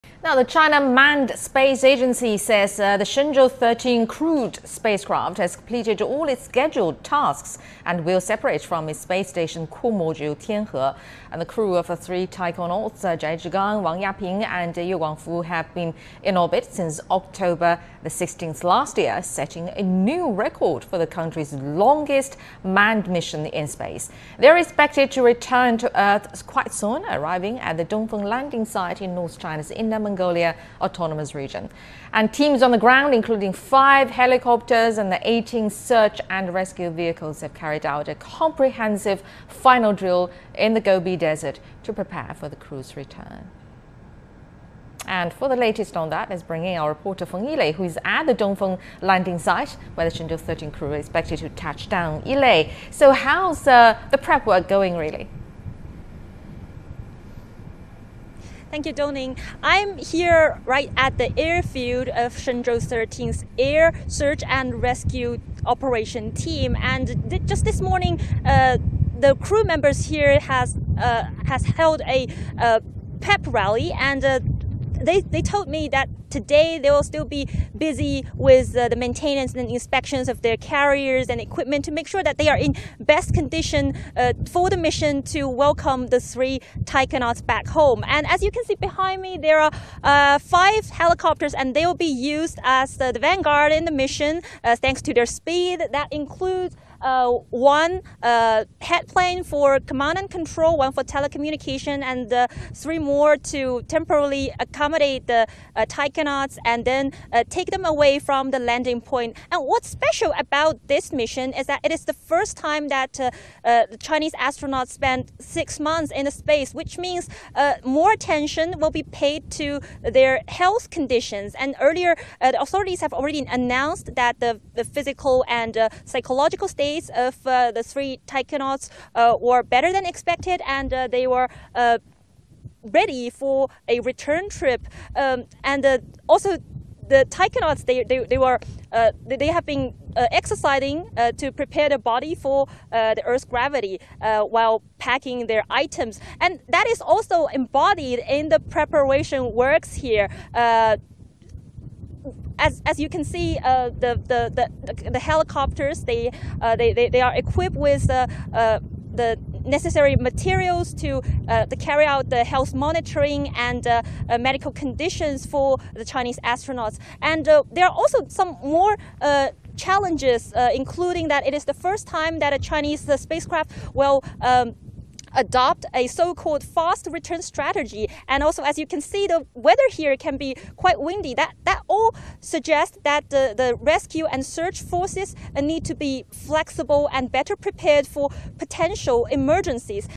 CHINA SPACE EXPLORATION: Shenzhou-13 to separate from space station core module
The China Manned Space Agency says the Shenzhou-13 crewed spacecraft has completed all its scheduled tasks, and will separate from the space station core module Tianhe. The crew of three taikonauts, Zhai Zhigang, Wang Yaping and Ye Guangfu, have been in orbit since October 16th last year, setting a new record for the country's longest manned mission in space. They are expected to return to Earth soon, arriving at the Dongfeng landing site in north China's Inner Mongolia Autonomous Region. Teams on the ground, including five helicopters and 18 search and rescue vehicles, have carried out a comprehensive final drill in the Gobi Desert to prepare for the crew's return.